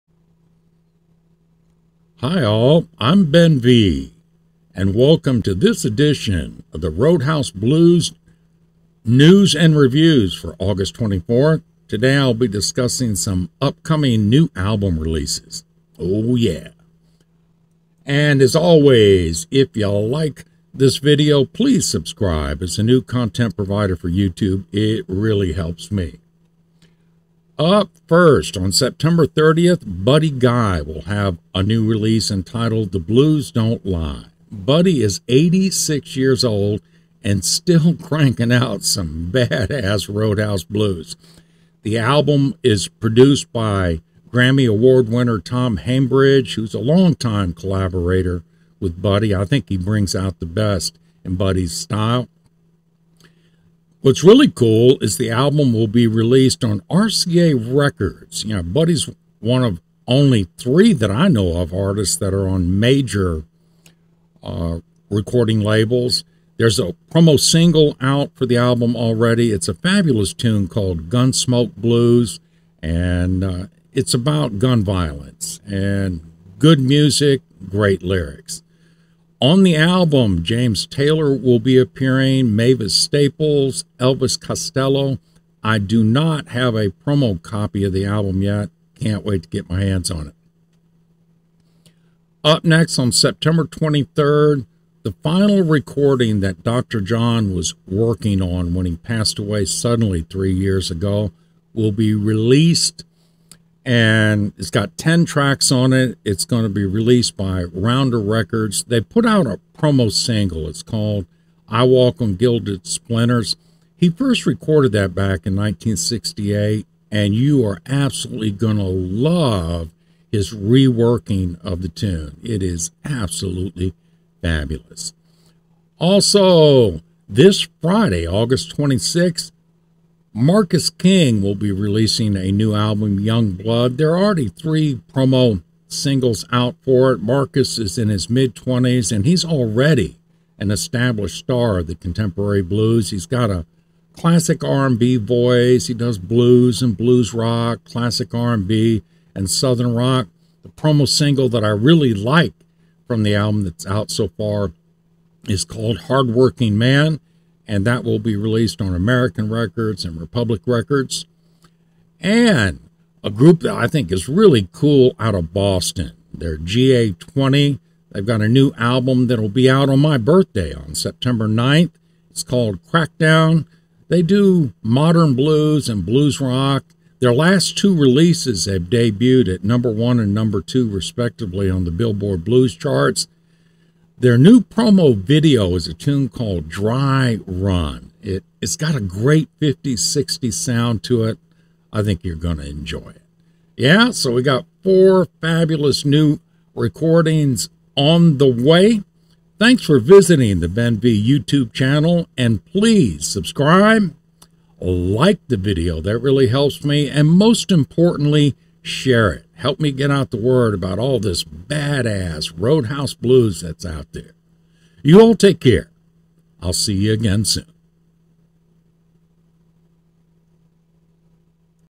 New Upcoming Roadhouse Blues Albums (August 24, 2022)
Some info on four great new roadhouse blues albums on the way from Buddy Guy, Dr John, Marcus King, and GA-20.